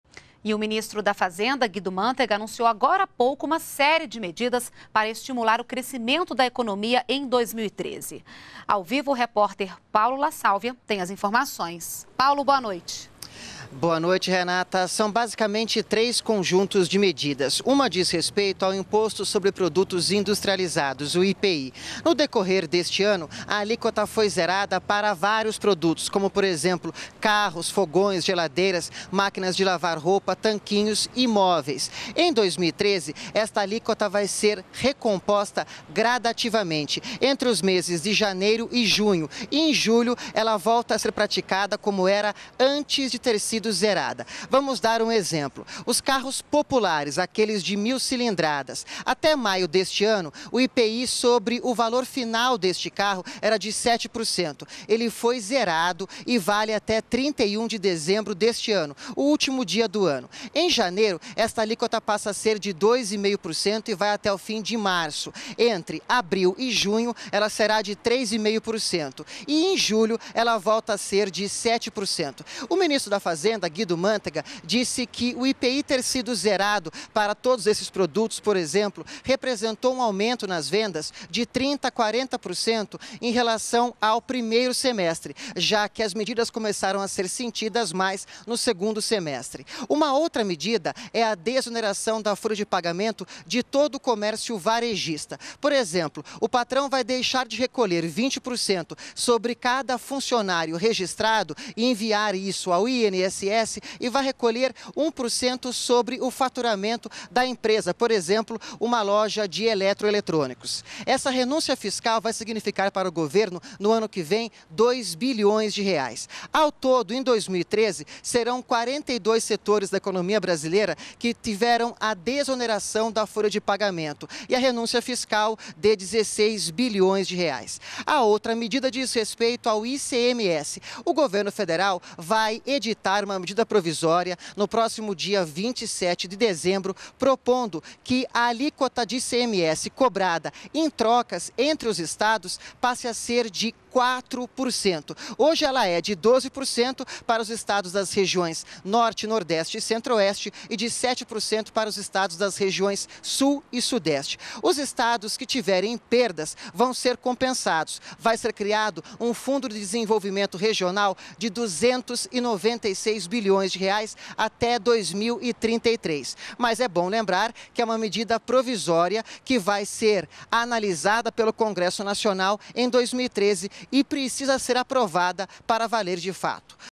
Guido Mantega anuncia medidas para estimular o crescimento da economia em 2013
NBR NOTÍCIAS - 19.12.12: O ministro da Fazenda, Guido Mantega, anunciou nesta quarta-feira (19) medidas para estimular o crescimento da economia em 2013. O conjunto de ações vai atingir o Impostos Sobre Produtos Industrializados (IPI), desoneração da folha de pagamento do comércio varejista e a unificação das alíquotas do ICMS.